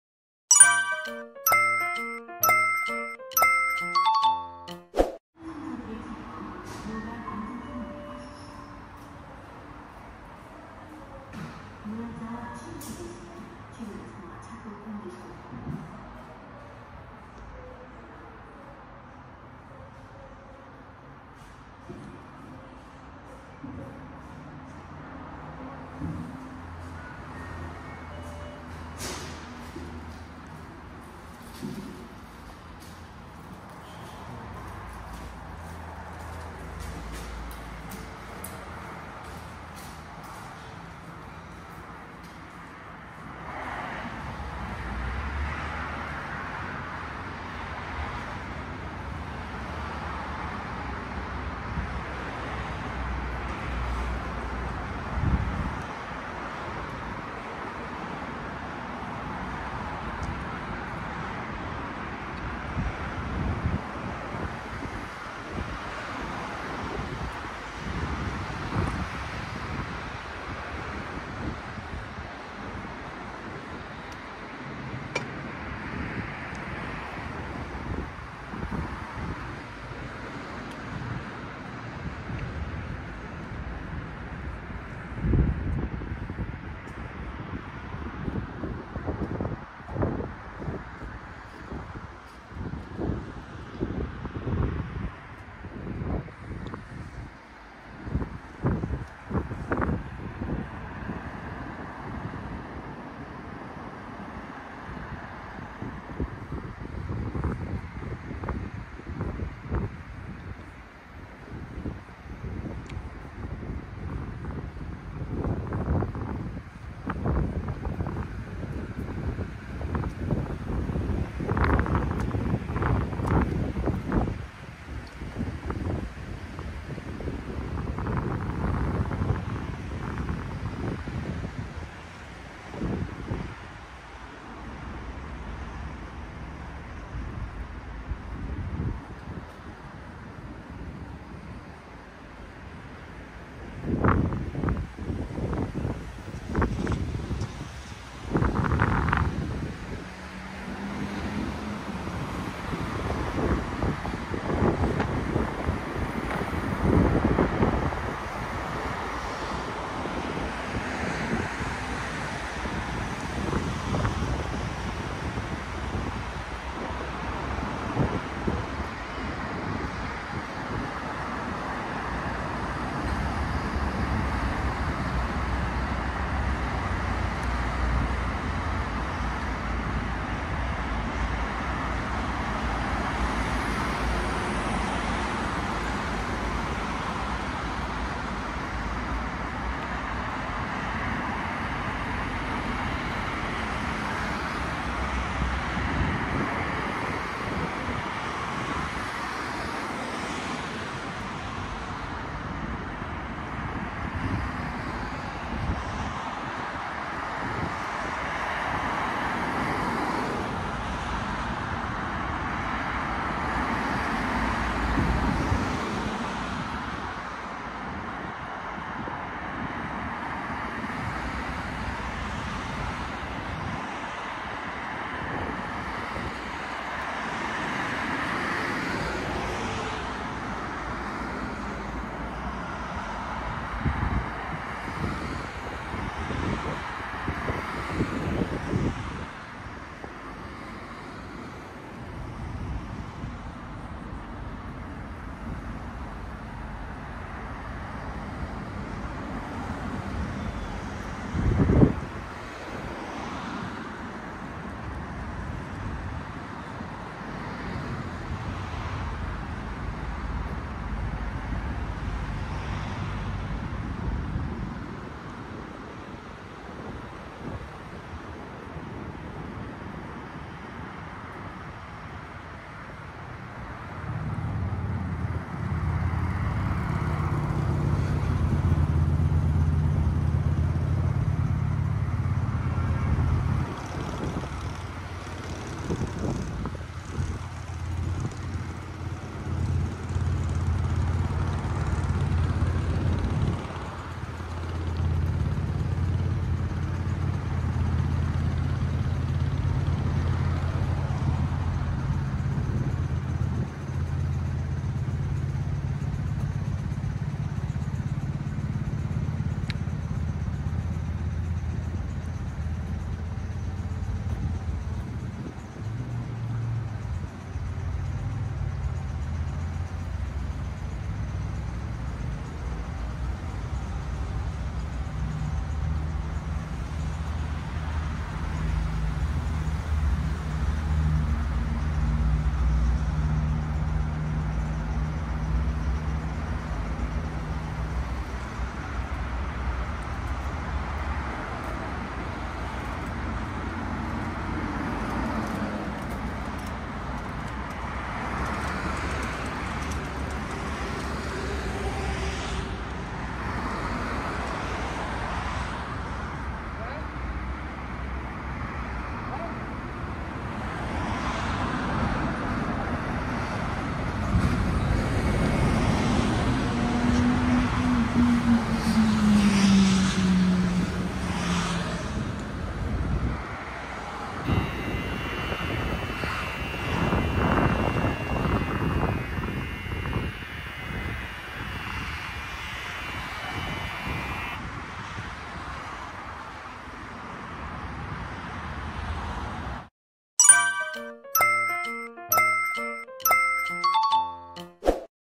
[해월당 호포점] 2호선 호포역 하차 후 도보로 가는법. [Haewoldang Hopo store]
주소: 경남 양산시 동면 양산대로 83 해월당 호포점